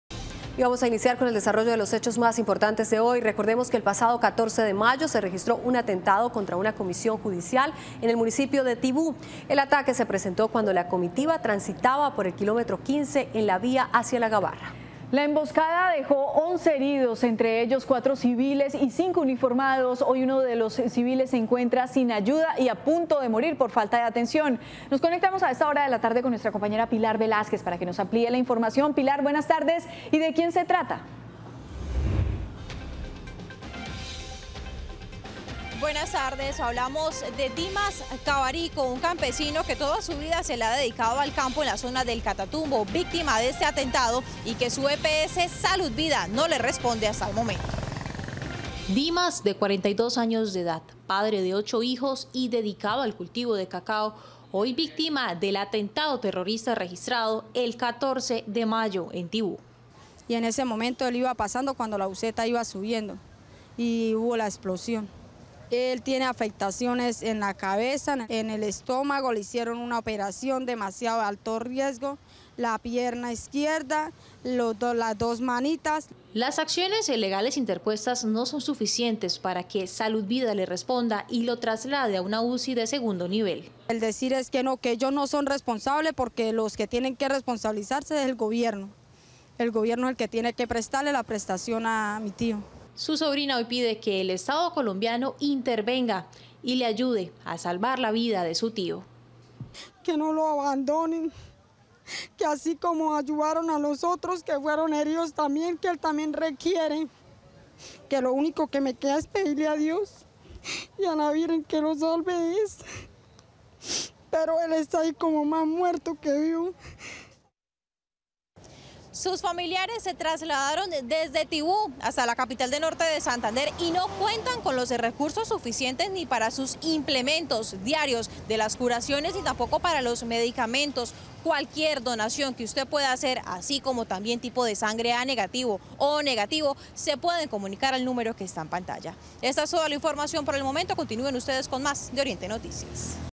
Presunta negligencia por parte de EPS en Norte de Santander
Familiares de un paciente herido con el carrobomba de Tibú, exigen a la EPS SaludVida atención integral para su ser querido